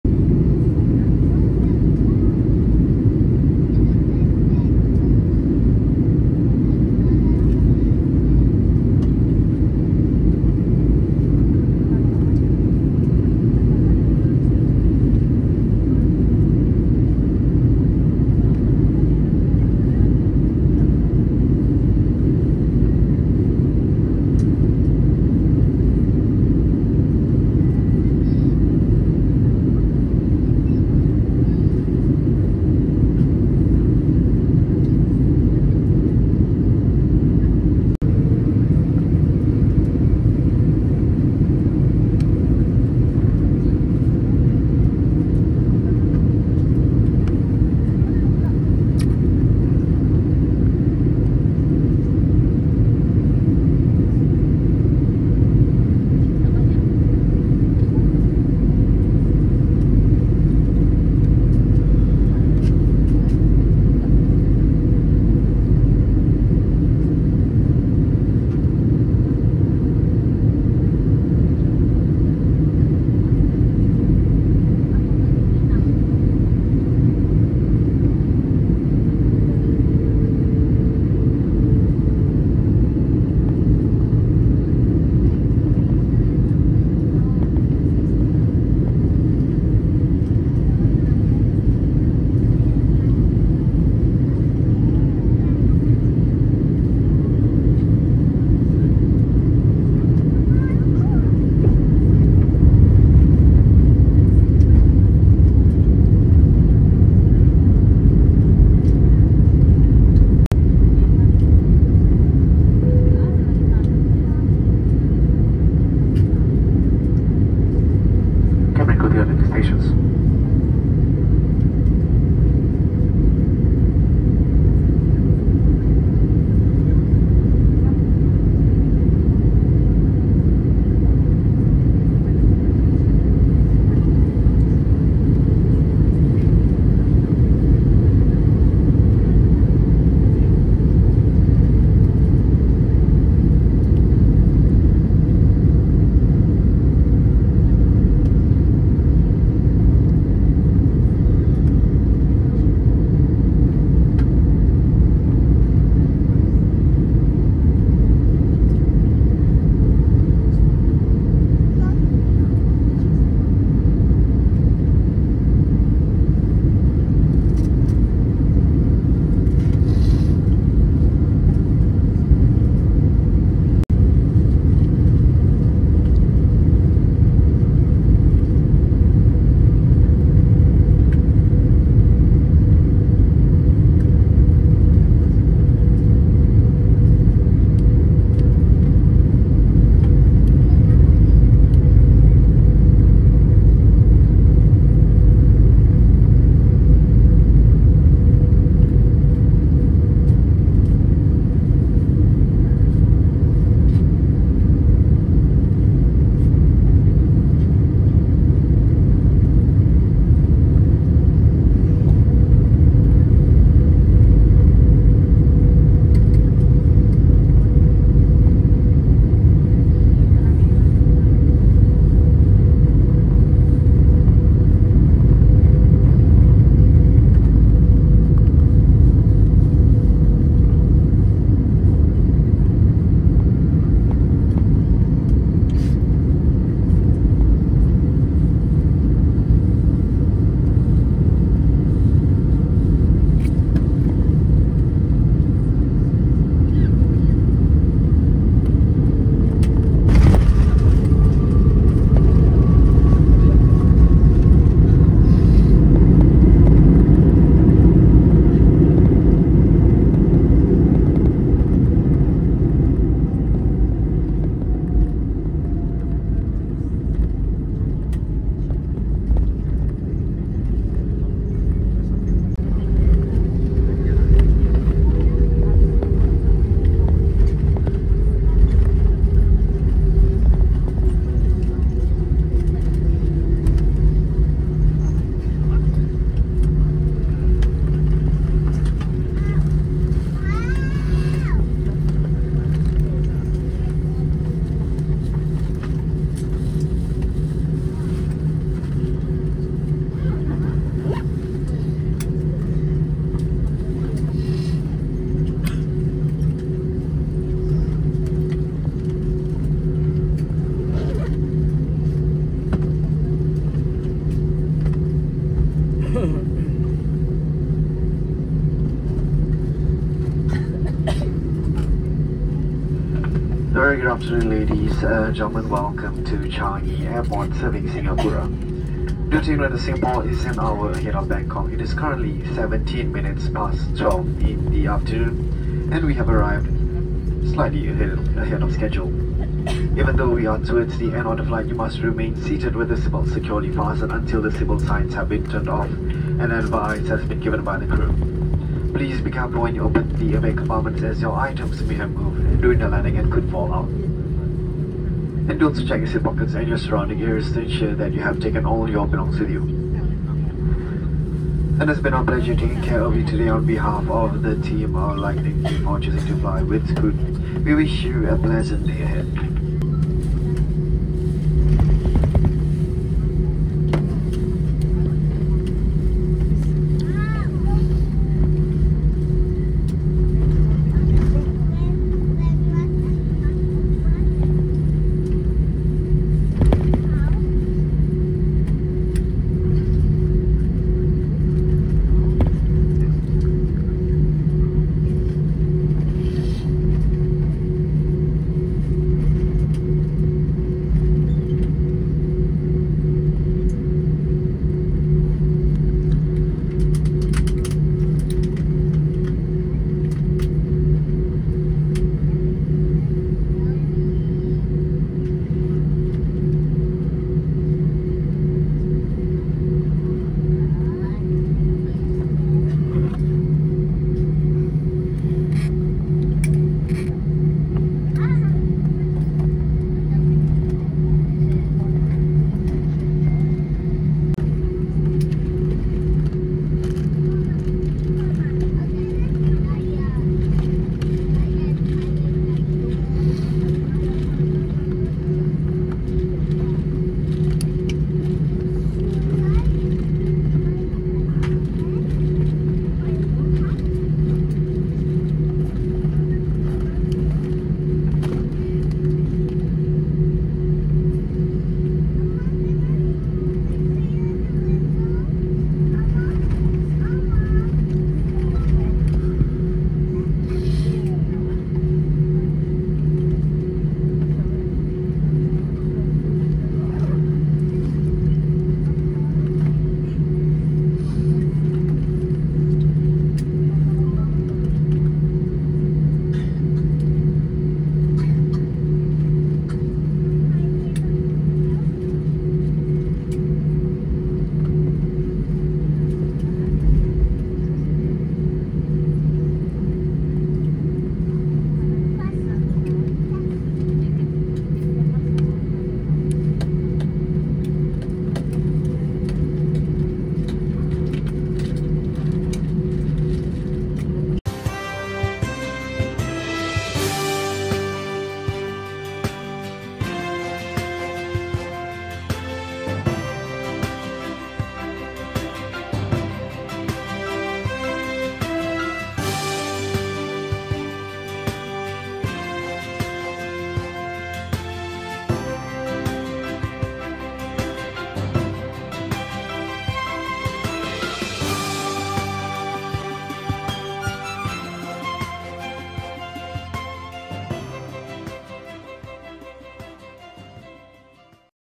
【シンガポール・チャンギ着陸】スクート609便 バンコク・スワンナプーム空港→シンガポール・チャンギ空港 Final approach to Changi Airport
2024年5月26日撮影、スクート609便 バンコク・スワンナプーム空港→シンガポール・チャンギ空港Airbus A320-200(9V-TRT)より撮影したシンガポール・チャンギ空港着陸の映像です。

Landing at Changi Airport, Singapore, taken by Airbus A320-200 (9V-TRT) from Bangkok Suvarnabhumi Airport to Changi Airport, Singapore, on May 26, 2024, by Scoot Flight 609.

Everyday Update!